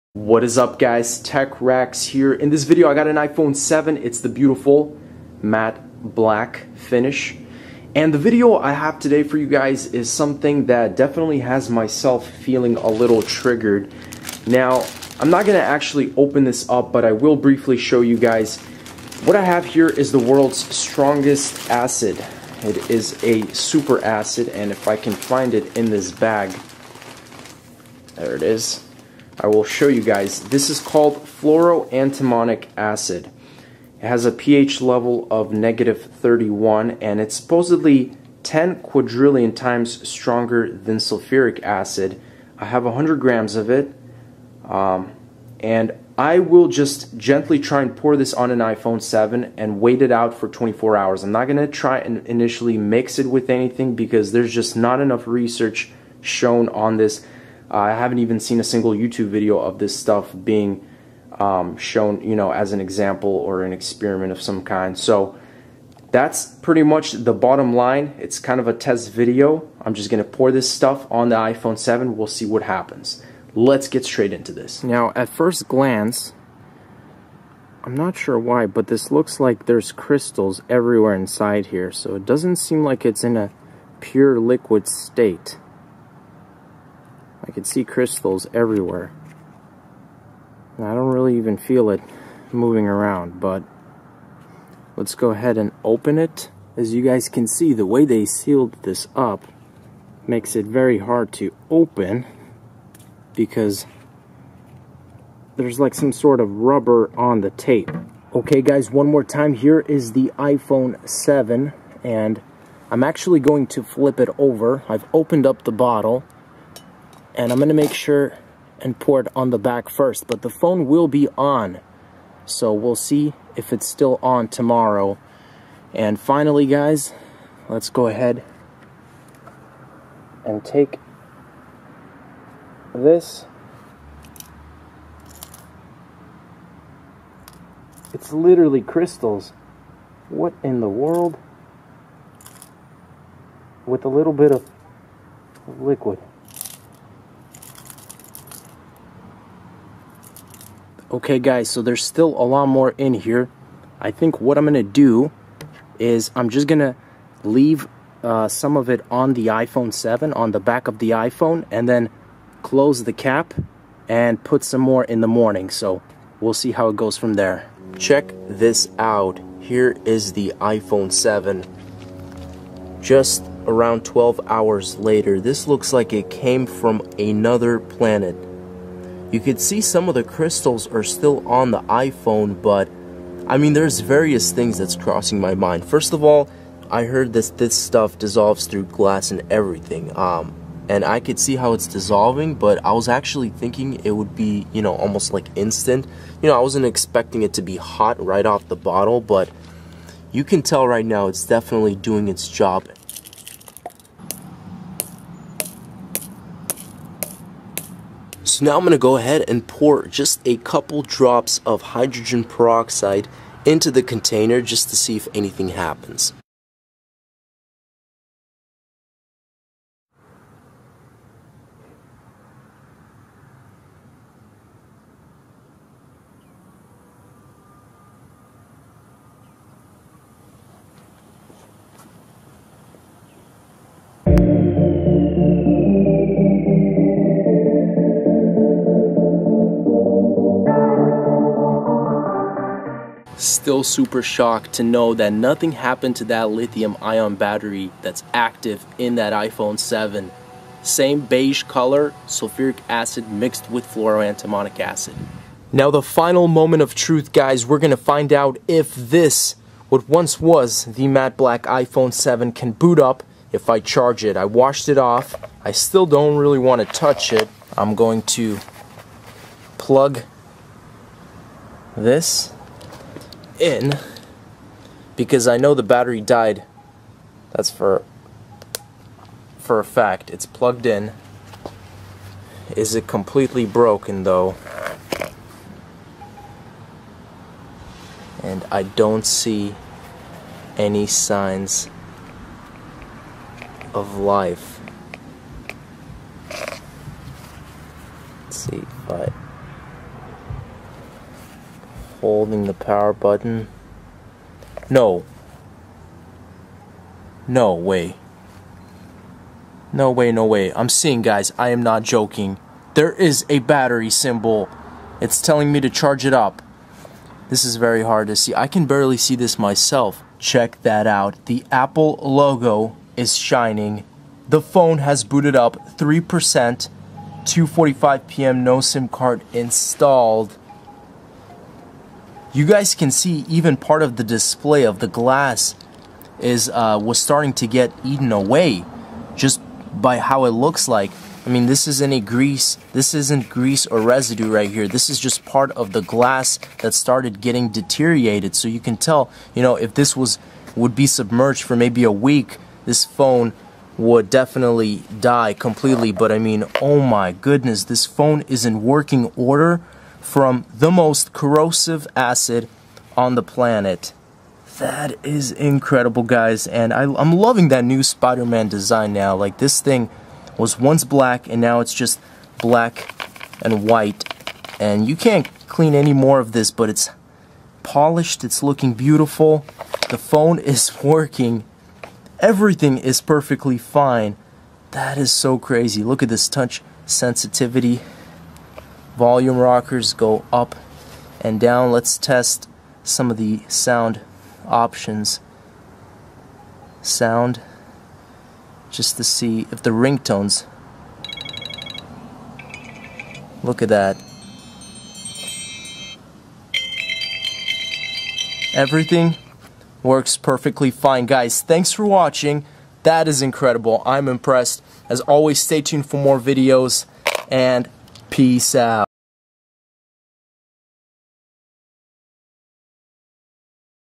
iPhone 7 vs World s Strongest Acid What Will Happen
How does Fluoroantimonic Acid react to an iPhone 7? I decided to find the answer!

iPhone 7
vs
World s Strongest Acid